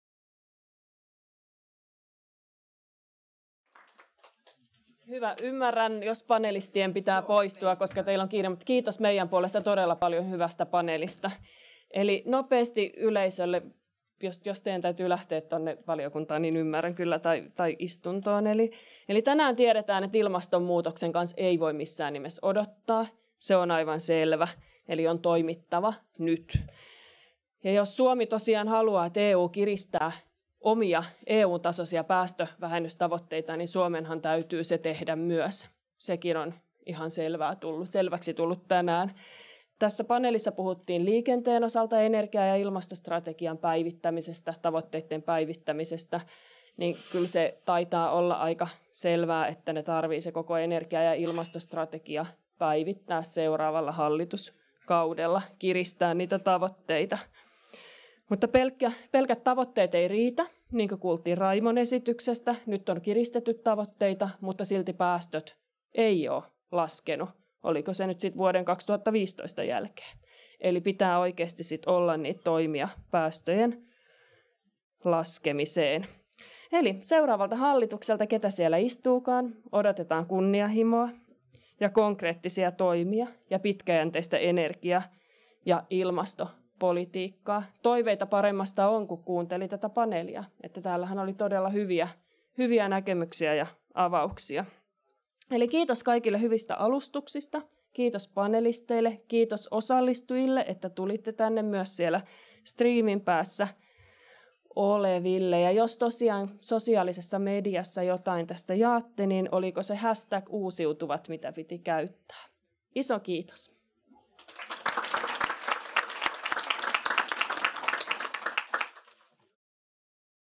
6 Yhteenveto - Kestävää kasvua uusiutuvilla
Anni Mikkonen, Tuulivoimayhdistys

Uusiutuvan energian yhdistykset Suomen Lähienergialiitto ry, Bioenergia ry, Pienvesivoimayhdistys ry, Suomen Biokaasuyhdistys ry, Suomen Lämpöpumppuyhdistys SULPU ry ja Suomen Tuulivoimayhdistys ry järjestivät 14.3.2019 yhdessä eduskunnan energiaremonttiryhmän kanssa tilaisuuden Kansalaisinfossa